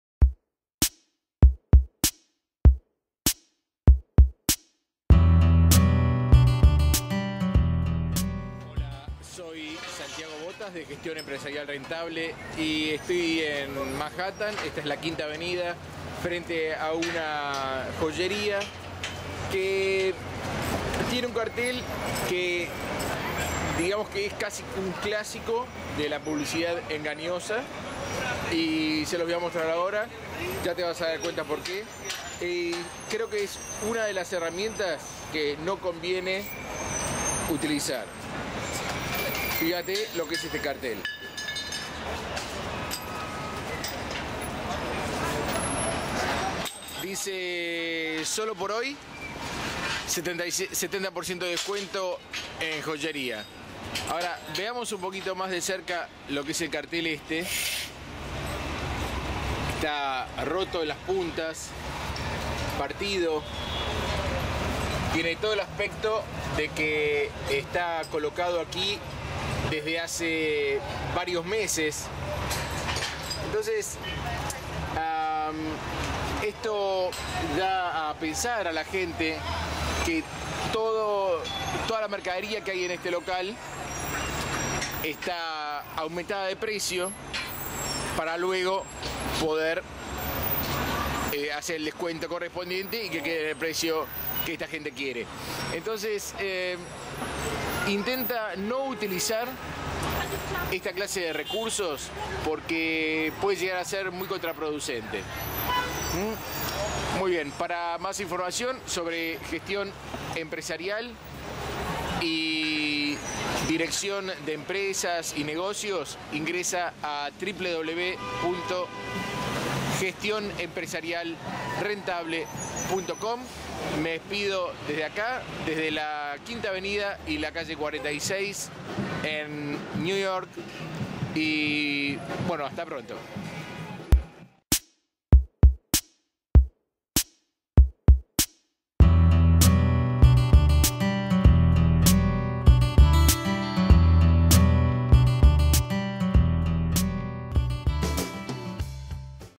Jamás Utilices Publicidad Engañosa para Aumentar tus Ventas
La publicidad engañosa es aquella que, de cualquier forma -incluida su presentación-, induce o puede inducir a error a sus destinatarios, puede afectar a su comportamiento económico o perjudicar o ser capaz de perjudicar a un competidor.
La ley también entiende por engañosa la publicidad que silencia datos fundamentales de los bienes, actividades o servicios, siempre que dicha omisión induzca al error a los destinatarios.
Por Santi Botas.